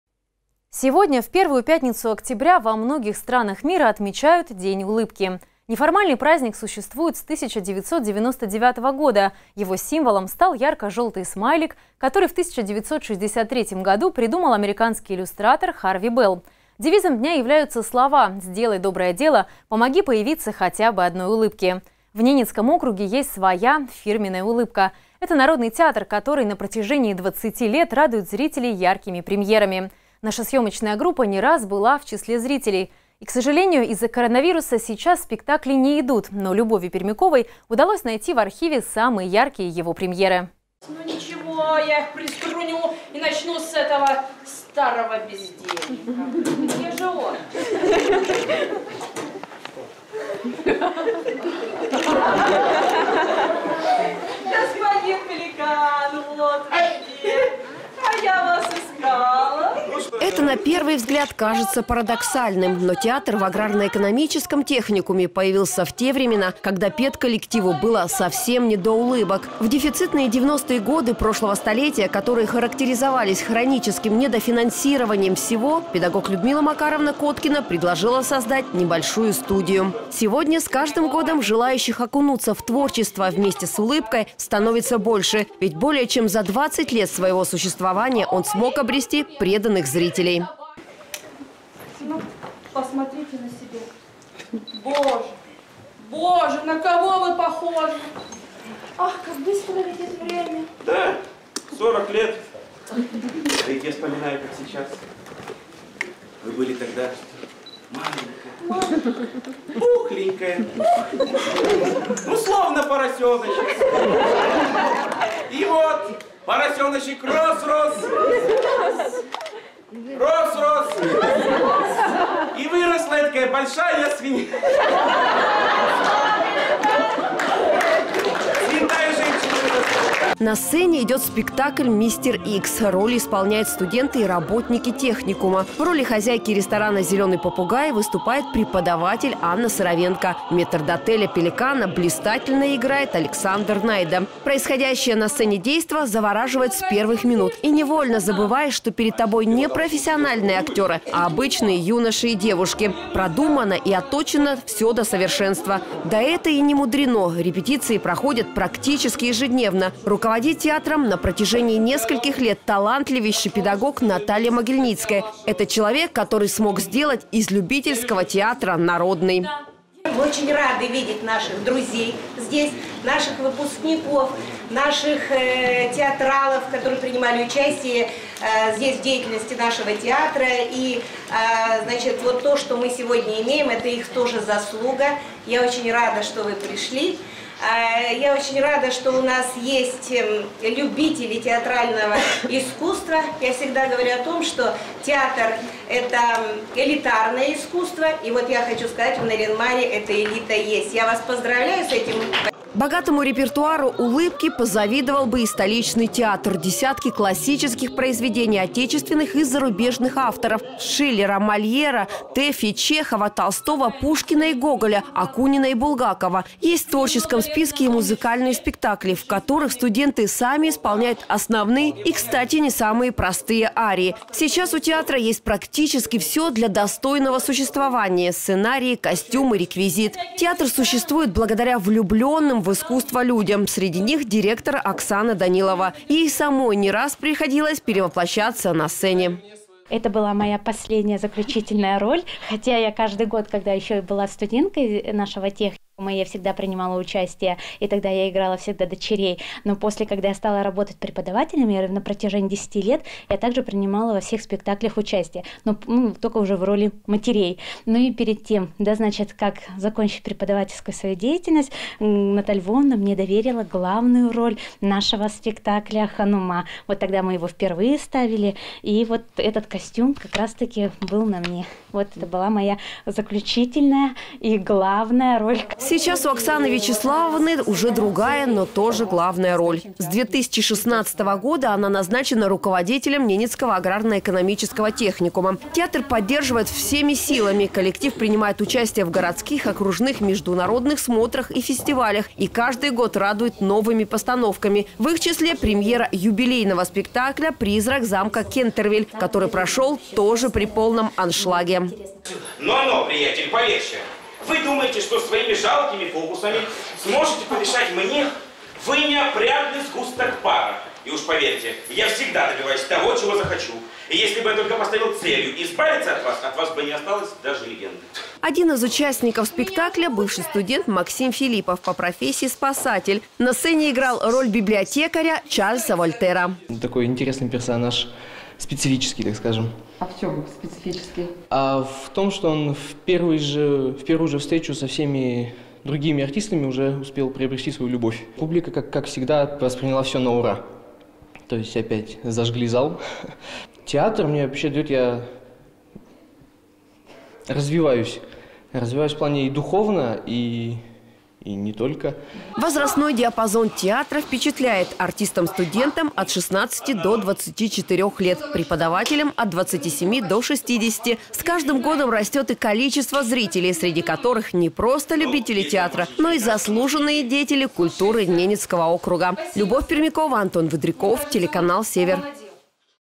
ВИЗИТНАЯ КАРТОЧКА НАЭТ-ТЕАТР "УЛЫБКА"!
Сегодня, в первую пятницу октября, во многих странах мира отмечают День улыбки. Неформальный праздник существует с 1999 года. Его символом стал ярко-желтый смайлик, который в 1963 году придумал американский иллюстратор Харви Бэлл. Девизом Дня являются слова: «Сделай доброе дело. Помоги появиться хотя бы одной улыбке». В Ненецком округе есть своя "фирменная" "Улыбка" - это народный театр, который на протяжении 20 лет радует зрителей яркими премьерами. Наша съемочная группа не раз была в числе зрителей. К сожалению из-за коронавируса сейчас спектакли не идут, но Любови Пермяковой удалось найти в архиве самые яркие его премьеры.